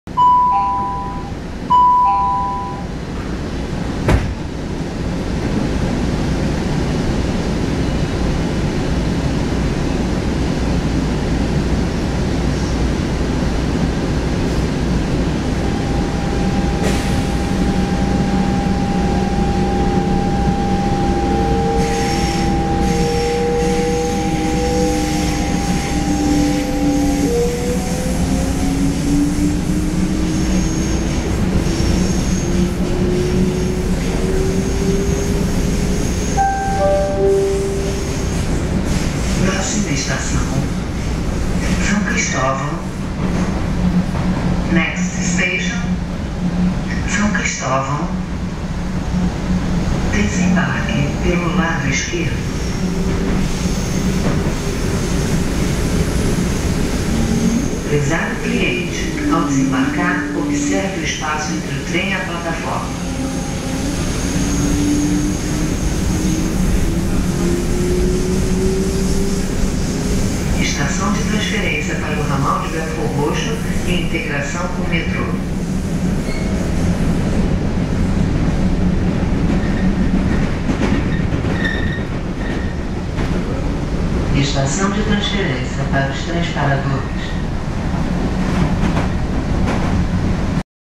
Riding the Supervia train in the suburbs of Rio de Janeiro between Triagem and São Cristovão
Riding the Supervia train in the suburbs of Rio de Janeiro between Triagem and São Cristovão - 30/04/2013